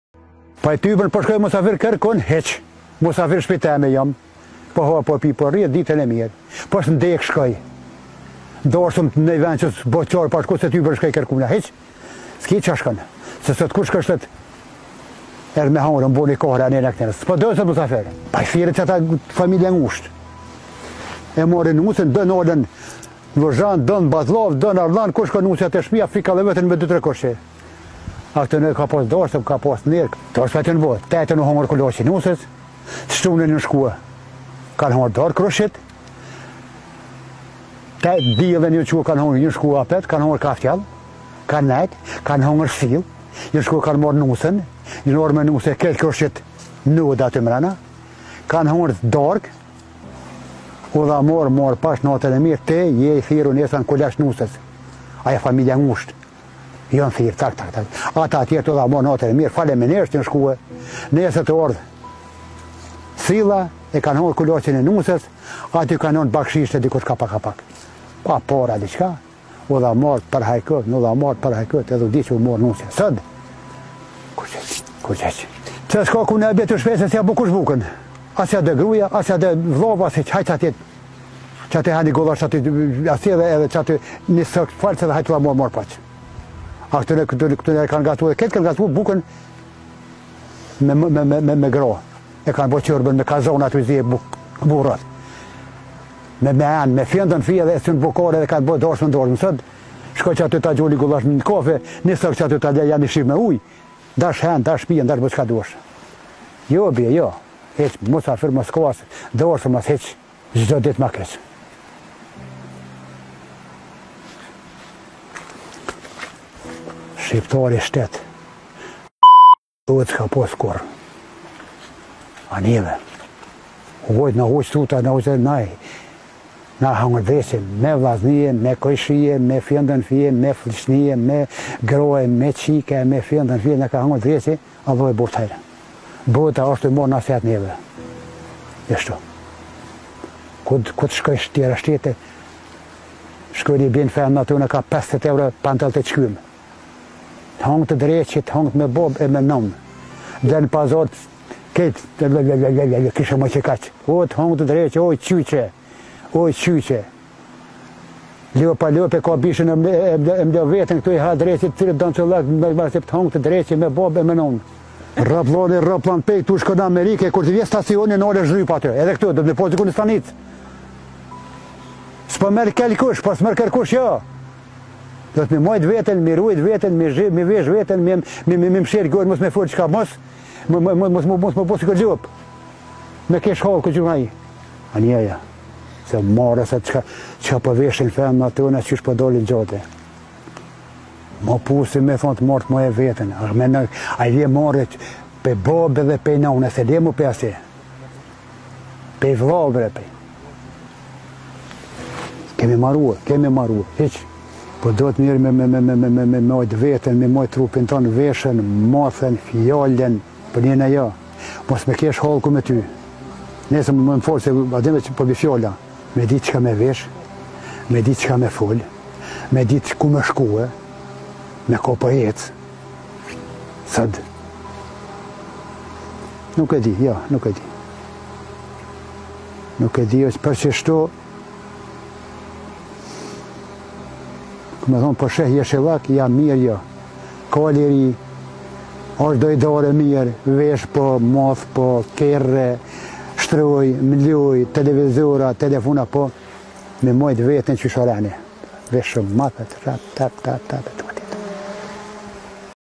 "Nuk ke çka shkon në dasma se sot ku te shkojsh thojnë erdhe me hangër e me boni kahre"
"Nuk ke çka shkon në dasma se sot ku te shkojsh thojnë erdhe me hangër e me boni kahre" -Gani Haliti nga fshati Orllan - Tregime nga katuni